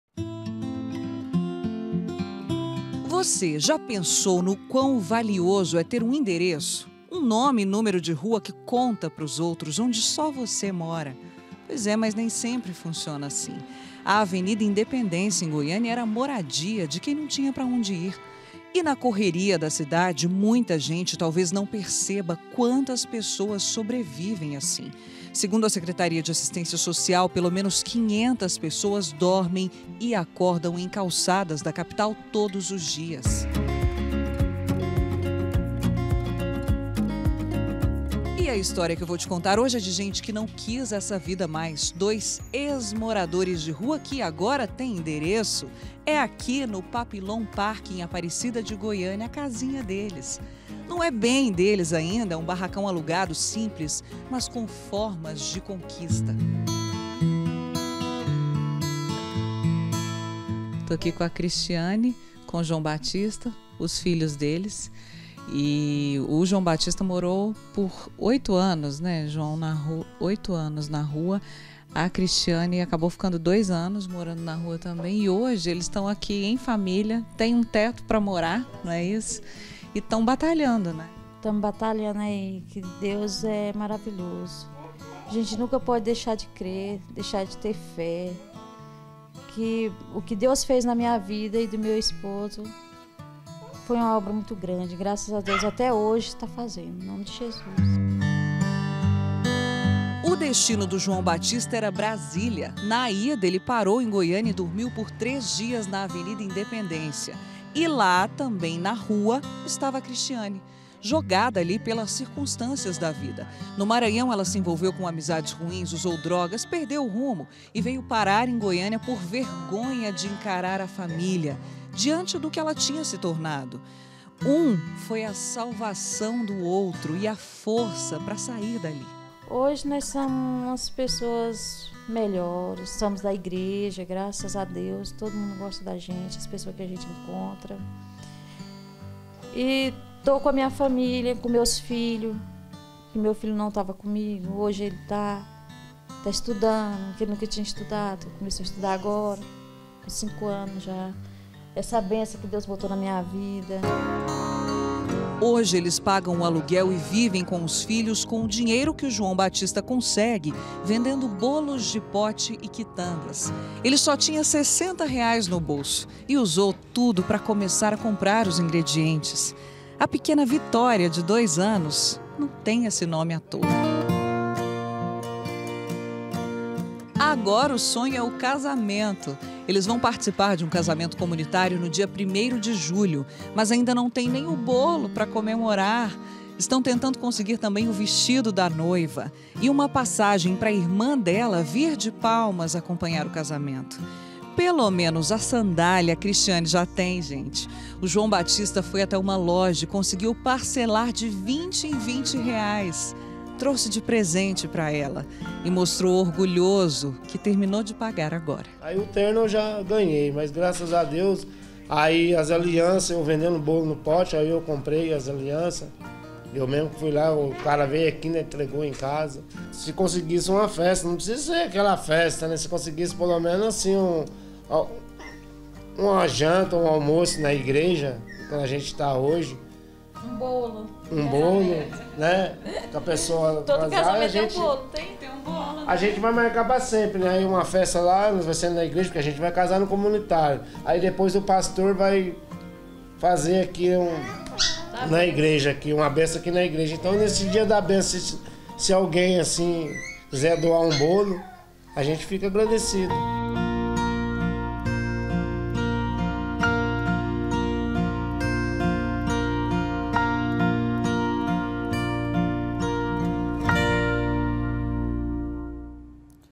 EX-MORADORES DE RUA QUEREM CASAR
Agora você vai conhecer uma história de superação. O João Batista e a Cristiane se conheceram enquanto moravam nas ruas e eram dependentes químicos. A força de vontade dos dois fizeram que a vida deles fosse transformadas. Hoje eles têm uma casa para morar e vivem a expectativa do casamento.